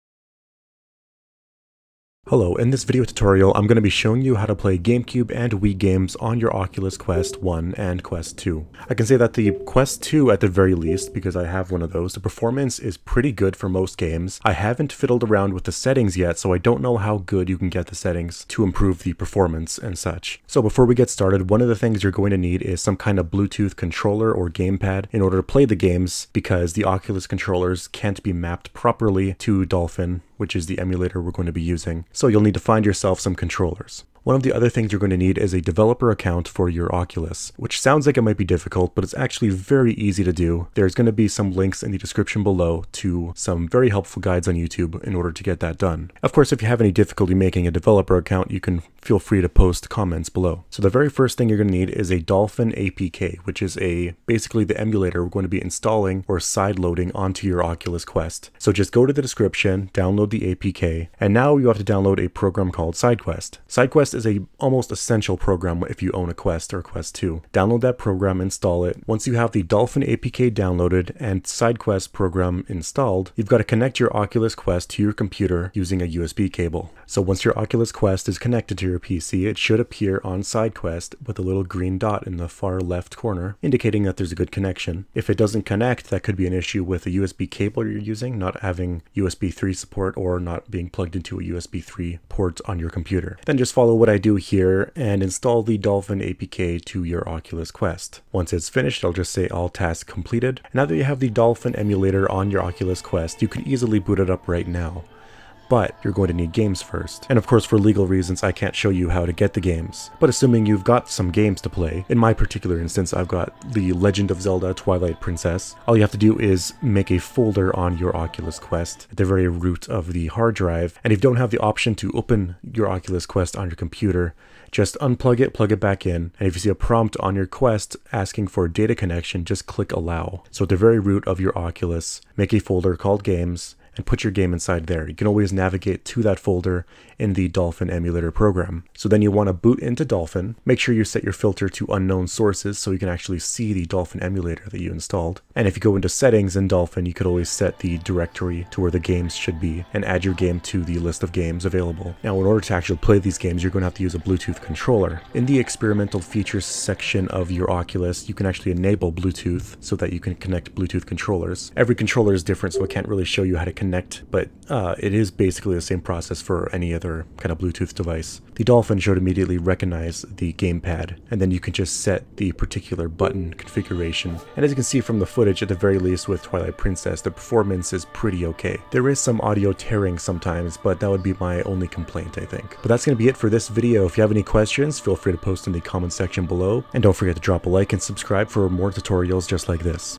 How to Play Gamecube Games on your Oculus Quest 2! [Tutorial/Guide]
Links:
-Sidequest